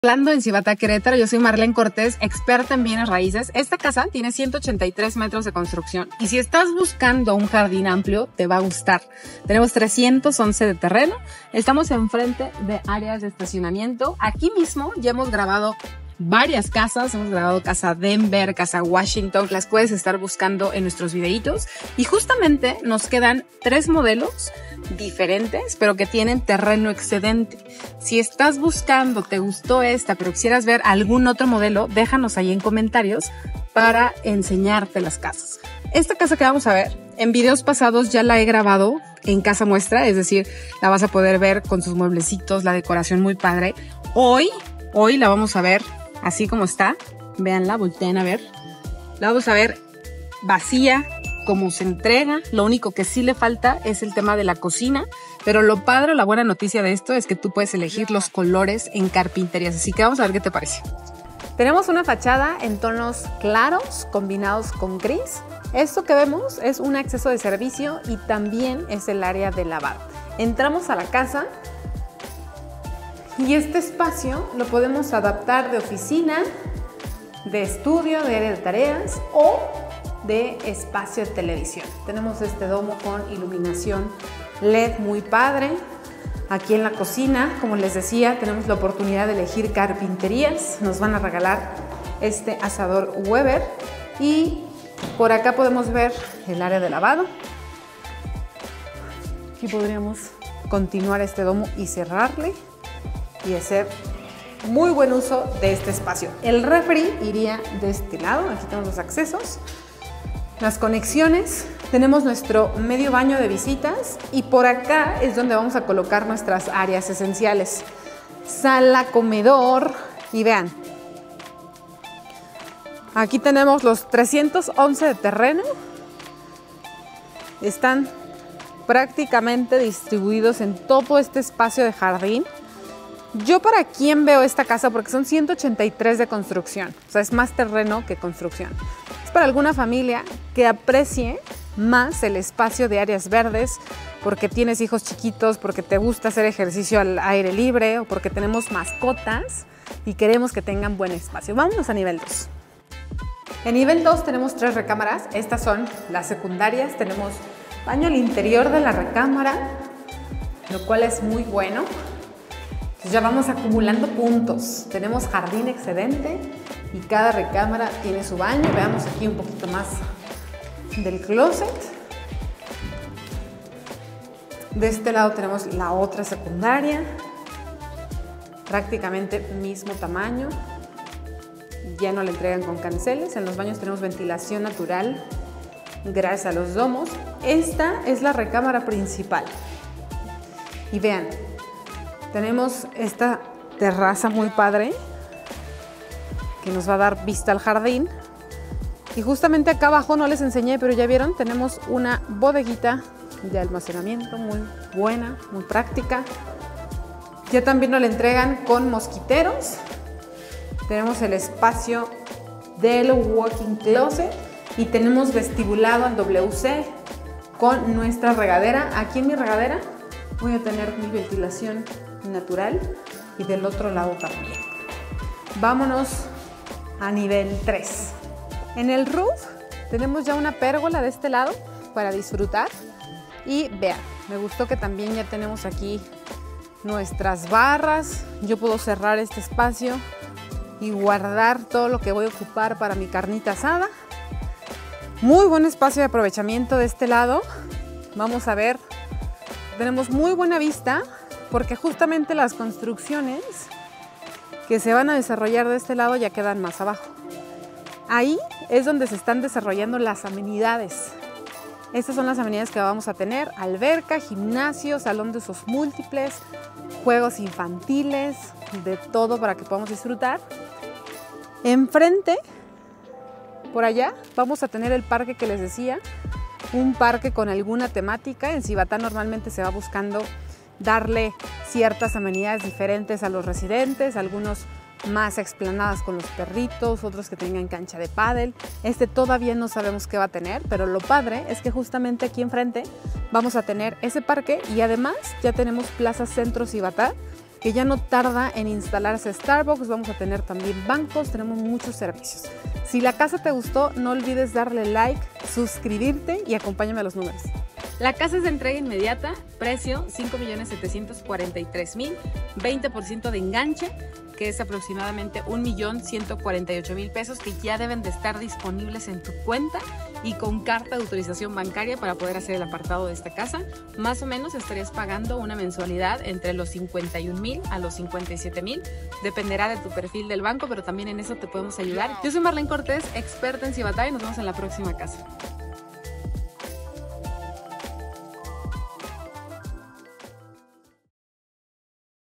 ¡Si buscas casa con jardín amplio, conoce CASA ORLANDO! | Zibatá, Querétaro
🏡✨ ¡Bienvenido a Casa Orlando, tu oasis de lujo y comodidad en el corazón de Zibatá! 💖 Esta espectacular residencia de 183 metros cuadrados de construcción y un amplio terreno de 311 metros cuadrados te espera con los brazos abiertos. ¡Y lo mejor de todo, la entrega es inmediata! 🚪💼

Con detalles personalizables, como la opción de elegir el color de la carpintería, Casa Orlando te brinda la libertad de crear el hogar de tus sueños. Su fachada en tonos claros y gris te da la bienvenida con elegancia y estilo. Además, cuenta con acceso de servicio y área de lavado para tu conveniencia. 🎨🏠

Desde el momento en que entras, te sorprenderá su espacio versátil en la entrada, perfecto para adaptarlo como oficina, sala de TV o estudio. El medio baño de visitas añade un toque de practicidad, mientras que el jardín sumamente amplio te invita a disfrutar del aire libre y la naturaleza. 🌳🪑

En el segundo nivel, encontrarás tres recámaras con baño completo, cada una con clóset de piso a techo y ventilación natural en los baños. La recámara principal te ofrece una terraza con vista al jardín, walk-in closet, WC vestibulado y regadera con ventilación natural, brindándote un espacio de relajación incomparable. 🛌🛁

Y si pensabas que eso era todo, ¡prepárate para el nivel 3! Aquí, podrás disfrutar de un roof con pérgola, barras preparadas para carne asada y una práctica bodega. Además, Casa Orlando te ofrece increíbles amenidades, como alberca, gimnasio, salón de usos múltiples y un futuro parque con canchas de pádel, ¡ideal para los amantes del deporte! 🏊‍♂️🏋️‍♀️🎾

💬 Déjame en comentarios si te gustaría conocer otras opciones